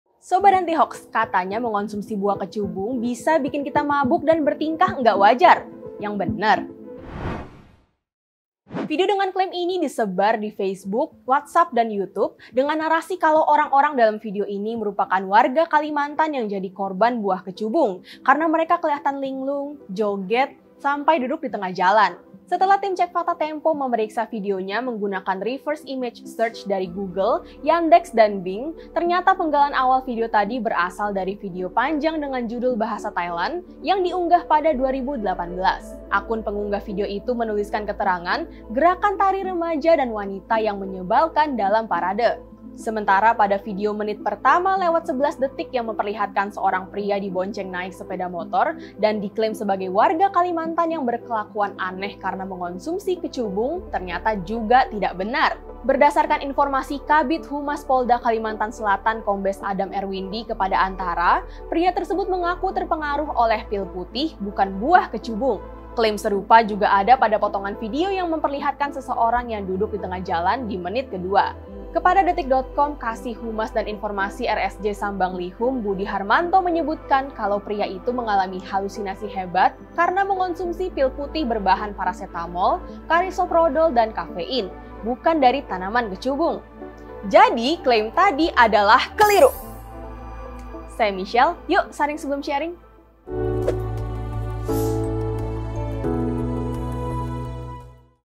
Warga Kalimantan Bertingkah Aneh karena Kecubung? | Cek Fakta Tempo
TEMPO.CO - Beredar sebuah video yang m enyebutkan bahwa beberapa warga Kalimantan terlihat linglung, menari, hingga duduk di tengah jalan setelah mengonsumsi buah kecubung.

Sobat antihoaks, yuk kita cek faktanya!

Laporan jurnalistik berkualitas dan enak dibaca, akses Tempo digital premium. Klik di sini

Dukung Channel YouTube TEMPO dengan join membership Pendukung TEMPO.